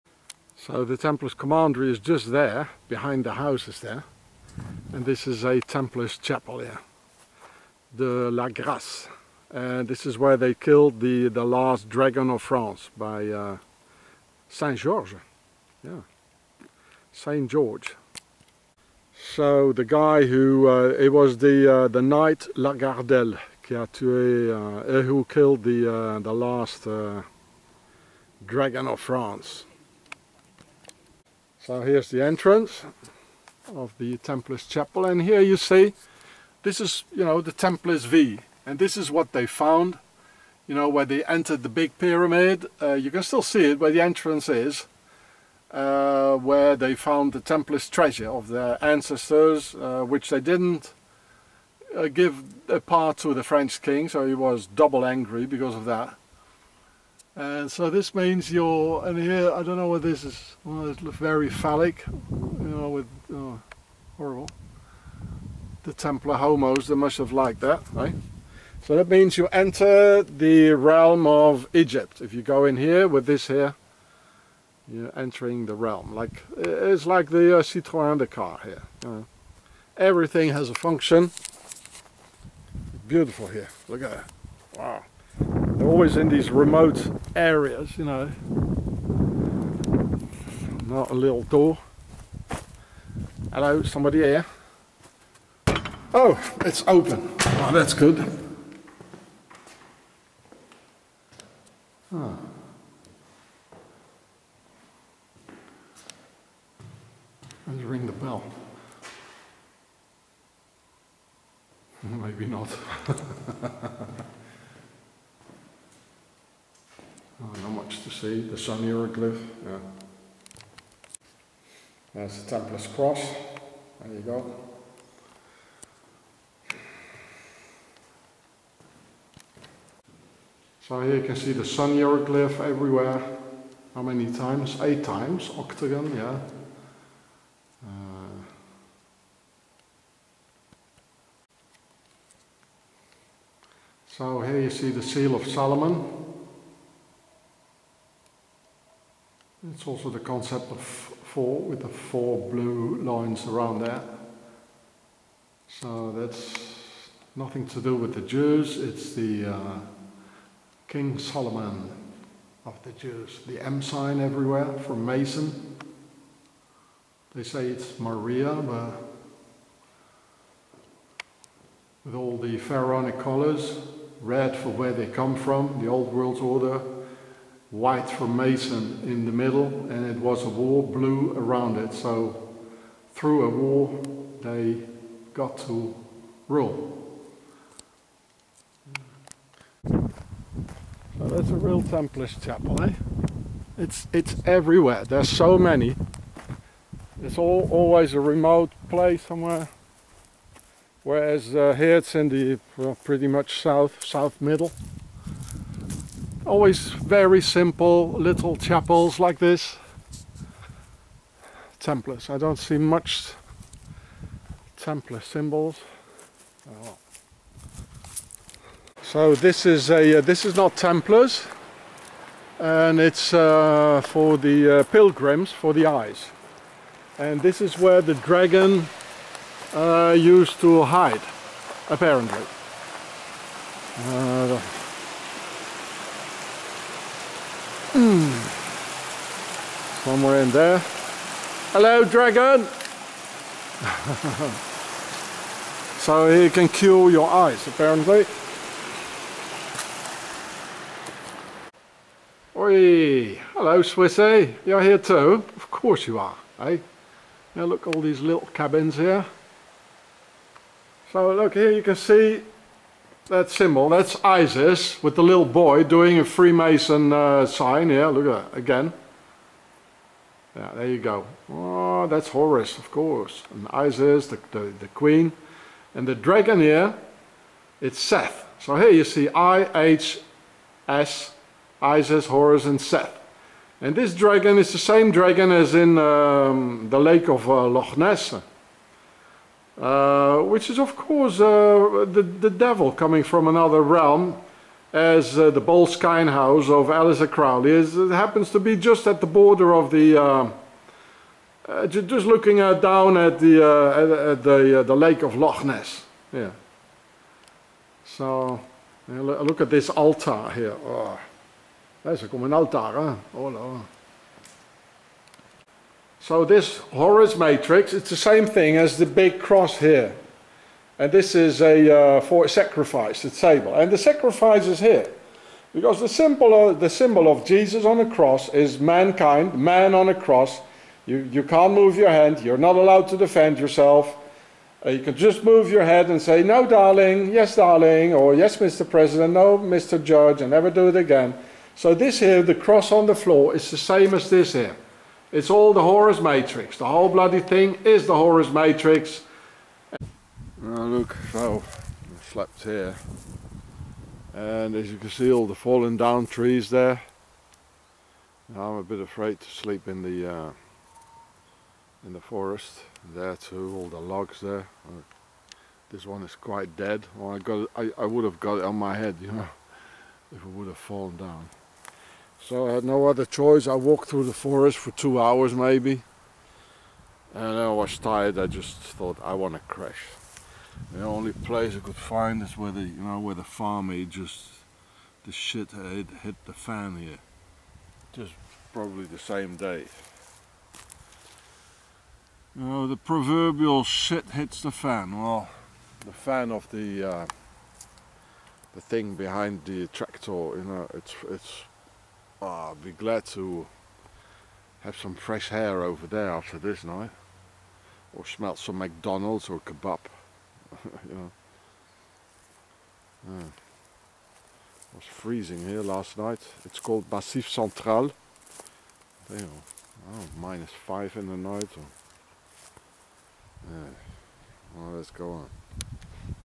Knights Templar Lagardelle killed Last Dragon of France in Chapel near Dragon's Cave
Mais l'histoire légendaire chrétienne commence avec le chevalier de Lagardelle (templier issu d'une famille de Caylus) qui, mandé par la population terrorisée par un dragon s'arme et va le tuer dans son antre.